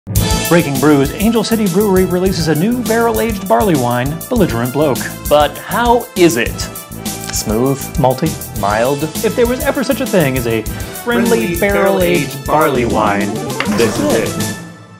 BREAKING BREWS - Belligerent Bloke from Angel City Brewery
Angel City Brewery has released a barrel-aged barleywine, Belligerent Bloke! How is it? Ben and Brian are ON IT.

In our new series BREAKING BREWS, we keep you up-to-date on the latest happenings around the SoCal brewing scene. If it's brown and bubbly, we're all over it!